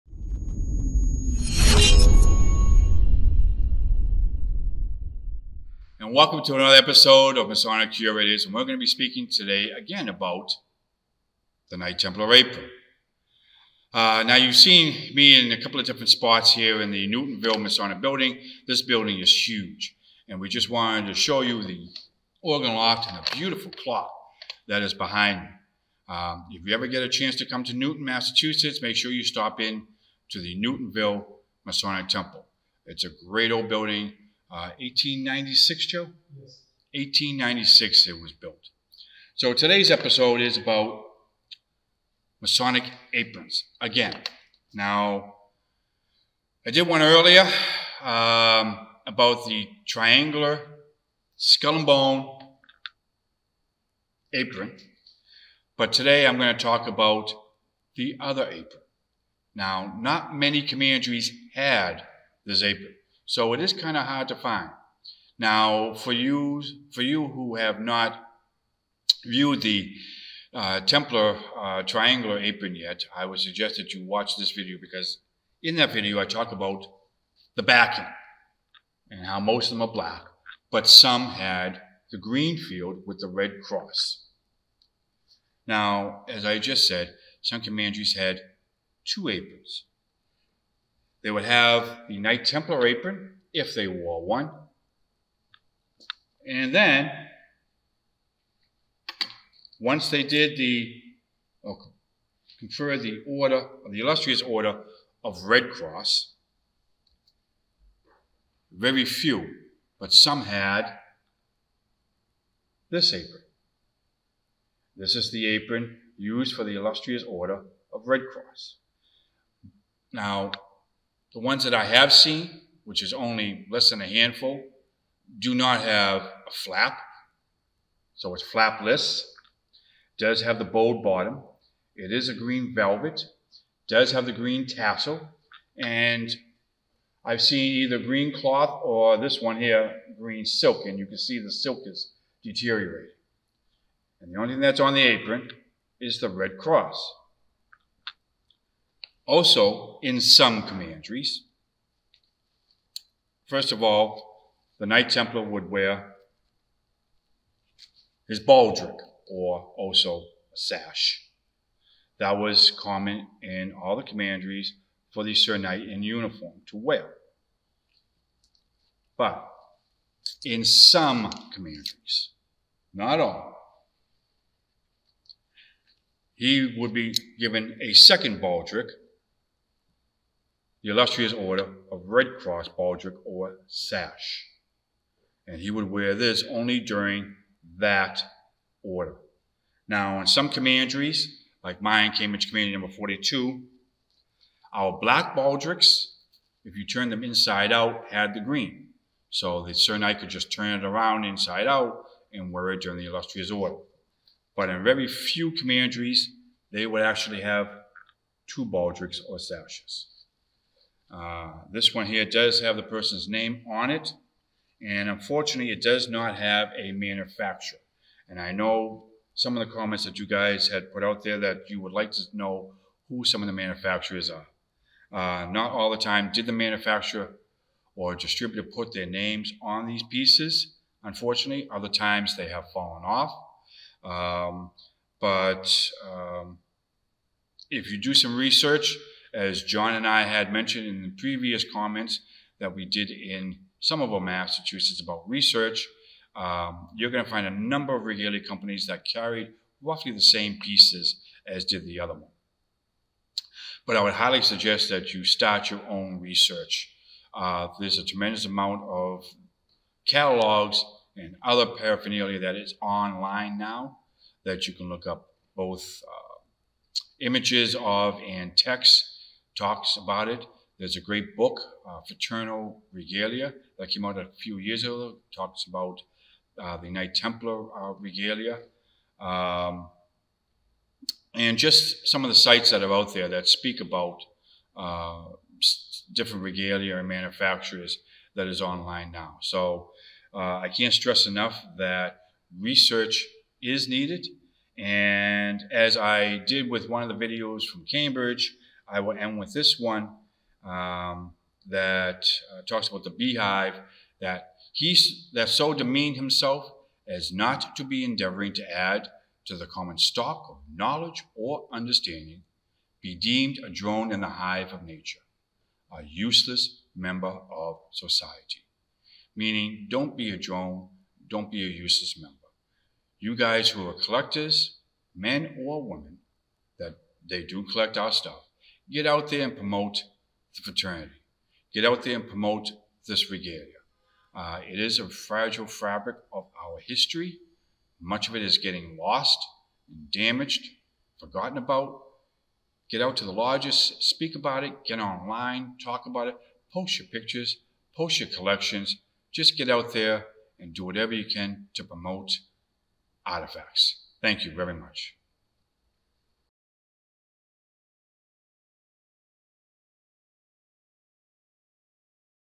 Masonic Curators - 098 - Illustrious Order of Red Cross Apron
In this episode, Keith Mackinnon tells us about a rare apron.